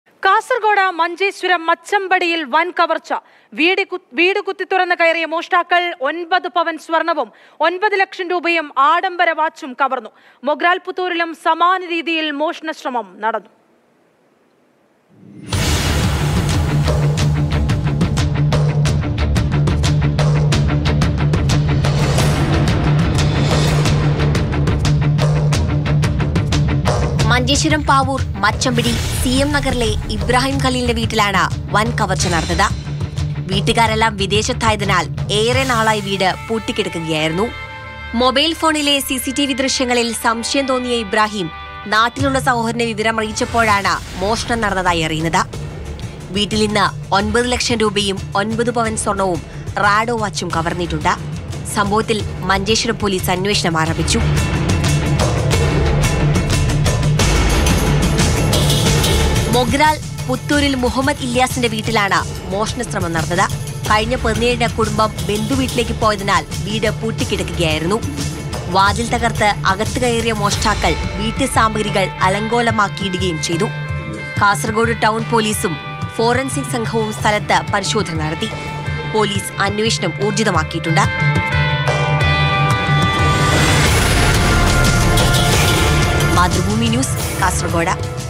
ഹെൽമെറ്റ് ധരിച്ച് ടോർച്ചുമായി കള്ളന്മാർ! പ്രവാസിയുടെ വീട് കുത്തിത്തുറന്ന് കവർച്ച | Theft
ഹെൽമെറ്റ് ധരിച്ച് ടോർച്ചുമായി കള്ളന്മാർ! പ്രവാസിയുടെ വീട് കുത്തിത്തുറന്ന് കവർച്ച, ആളില്ലാത്ത മറ്റൊരു വീട്ടിലും മോഷണശ്രമം, നാടുവിലസി കള്ളന്മാർ...

Happy viewing!